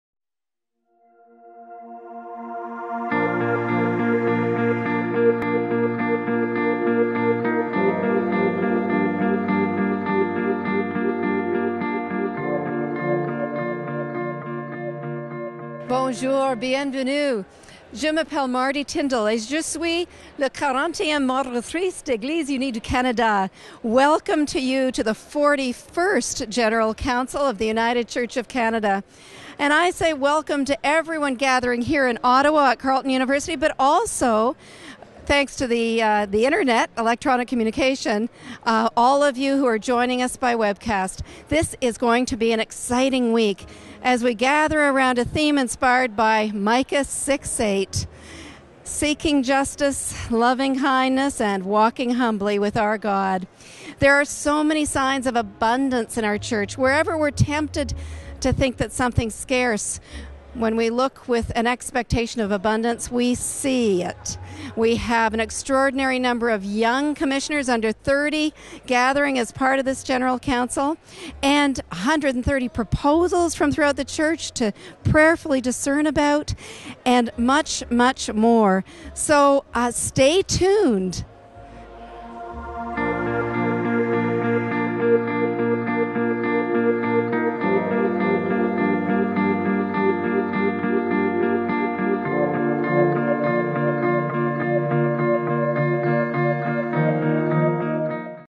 Moderator's Welcome
Moderator Mardi Tindal welcomes you to the 41st General Council in Ottawa.